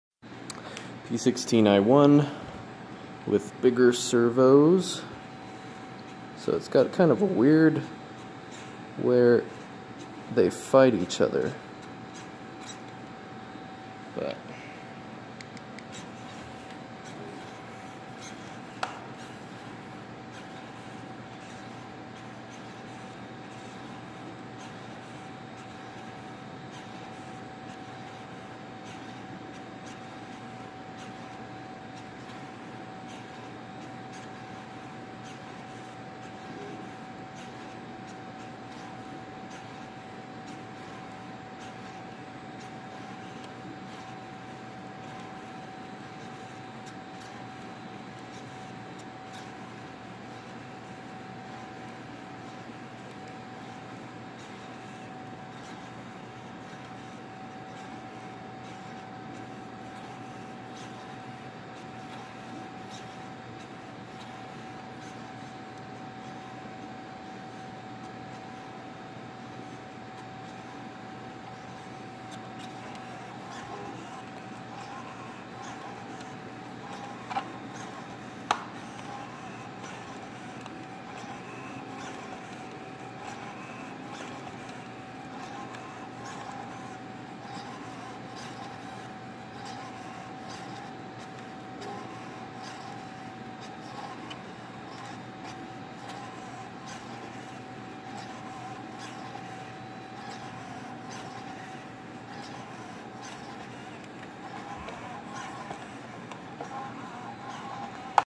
MTE P16I1 static test 5245 servos
( Morphing Trailing Edge: P - Prototype, I - Iteration )
Static test. This model has servos mounted near 0.23 x/c. The servos are set up to test aileron control. This model consists of a 1" stiff rib, 5" of morphing material, and another 1" of stiff rib. This is done to examine the gradient at the trailing edge. Using HS-5245MG servos. This model specializes in that the regular model has a lip actuated only on the lower surface, whereas this model has both the lower and upper surface lips actuated.

I conduct this research as a graduate student researcher in the USU AeroLab.